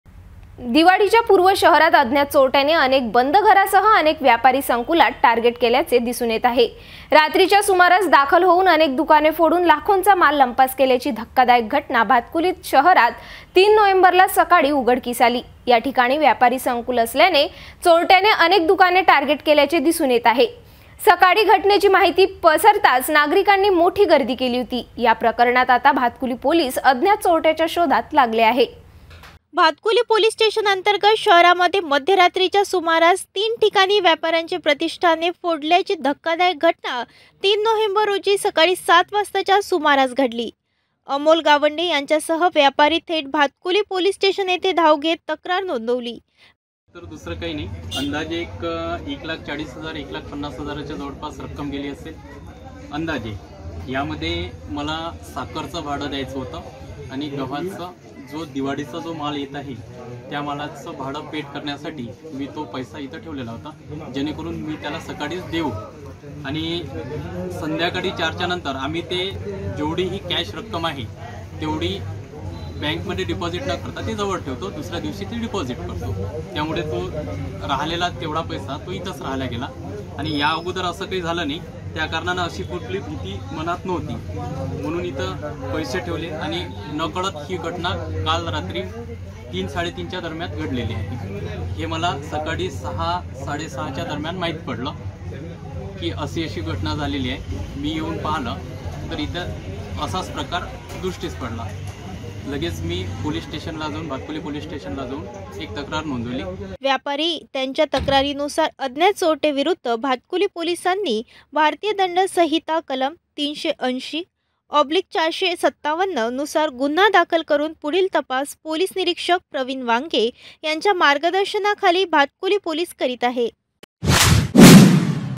भातकुली शहरातील दुकाने फोडून लाखोंचा ऐवज चोरी, गावात भीतीचे वातावरण
मध्यरात्रीच्या सुमारास एकामागे एक दुकाने फोडली
मात्र सीसीटीव्ही कॅमेरात चोरटे झाले कैद